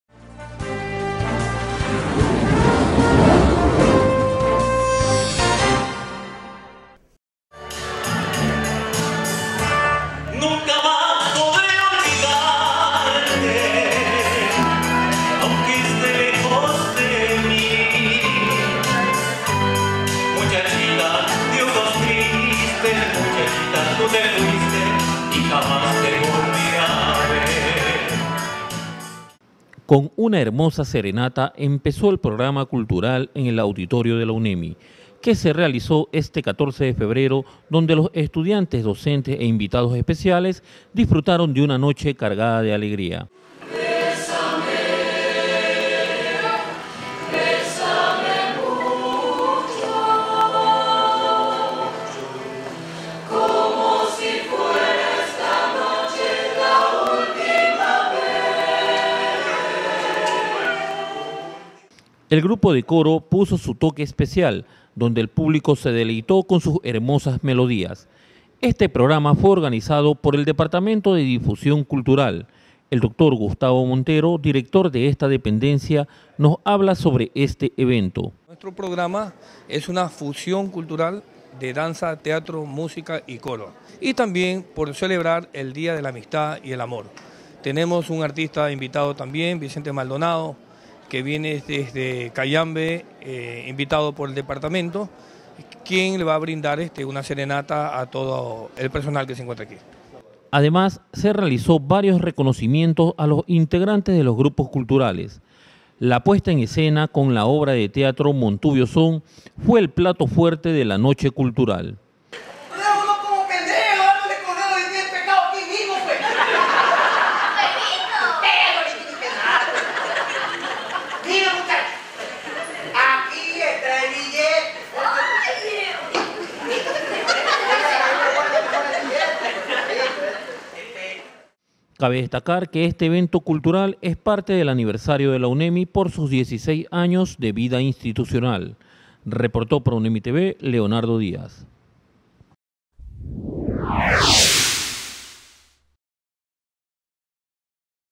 Noche cultural en UNEMI
El Departamento de Difusión y Eventos Culturales de la Universidad Estatal de Milagro (UNEMI) organizó una noche cultural con motivo del mes de aniversario de la alma mater milagreña.